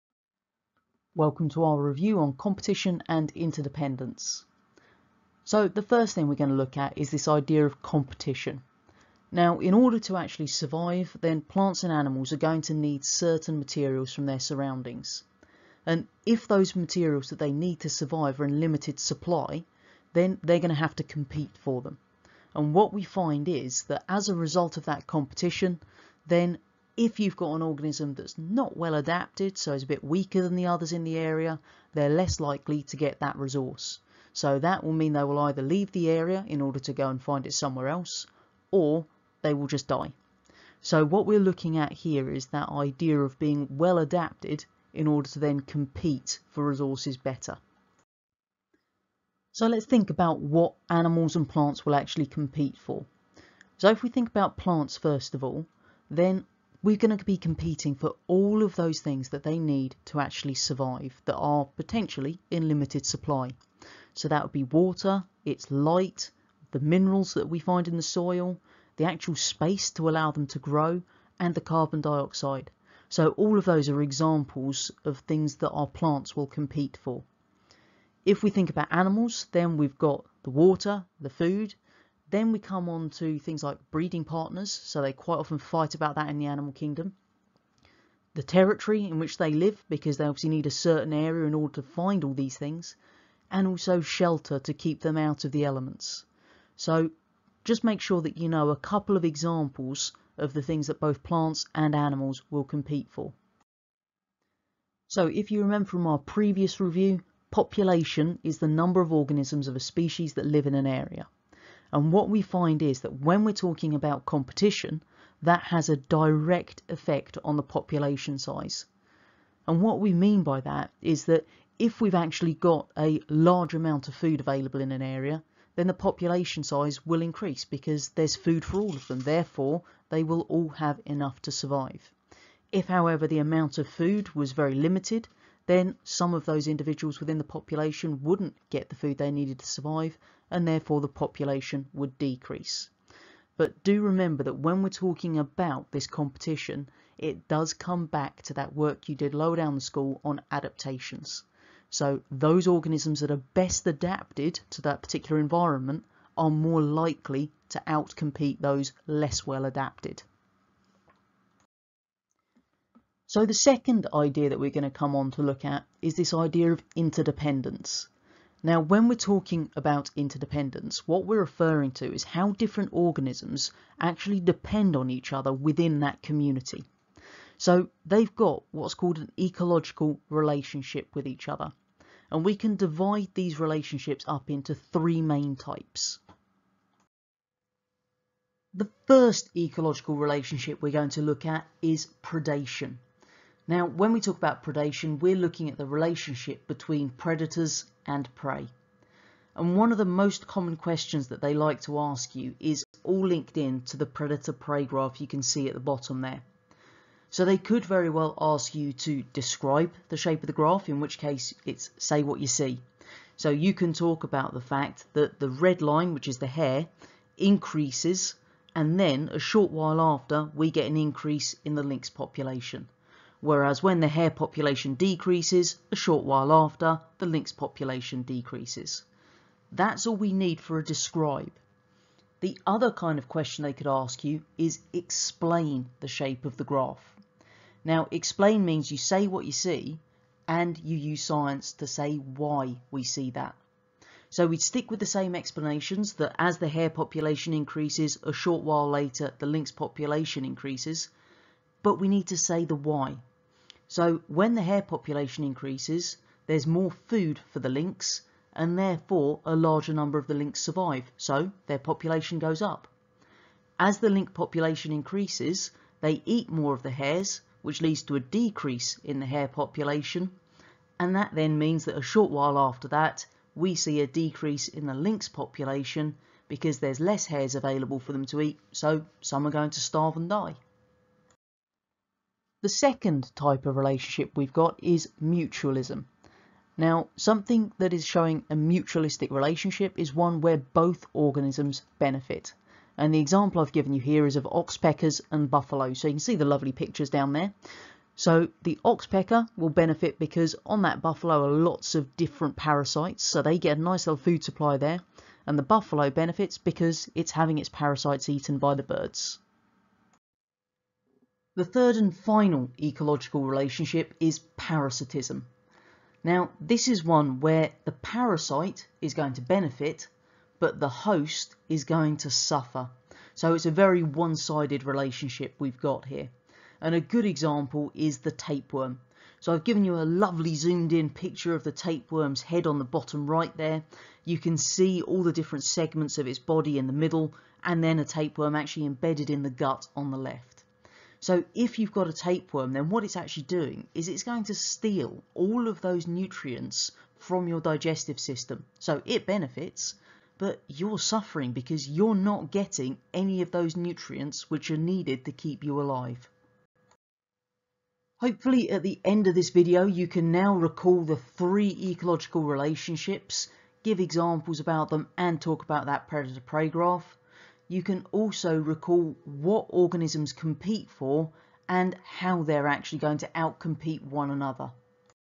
OCR Gateway A B4.1.3 - Competition and Interdependence
OCR Gateway A Biology (New Specification - First exams in 2018) revision summary on B4.1.3 - Competition and Interdependence for the higher and foundation tier.